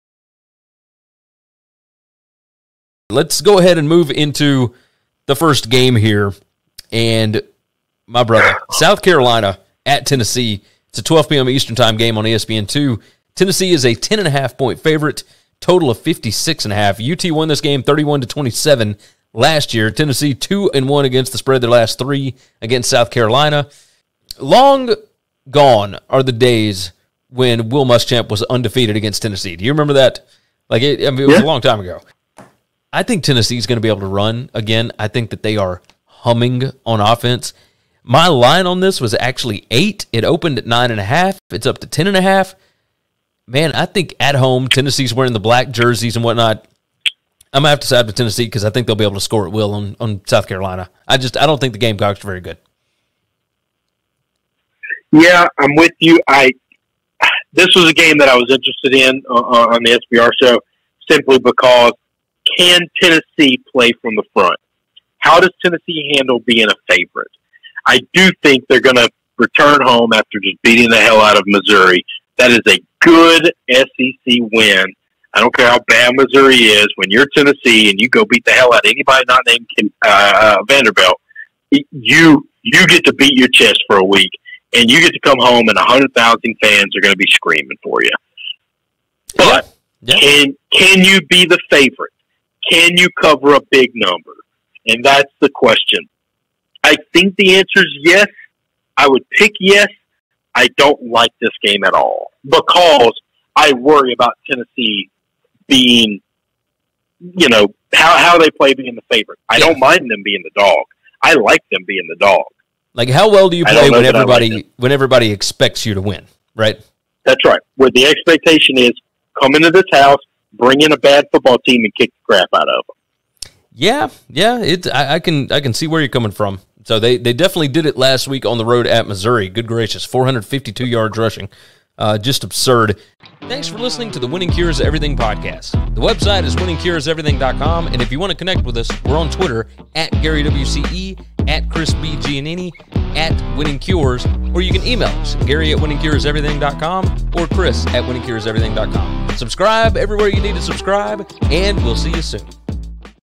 South Carolina vs Tennessee Picks Against the Spread 2021 College Football
Gary & Chris give a quick preview, predictions & their picks against the spread (ATS) for the South Carolina vs Tennessee college football matchup this weekend!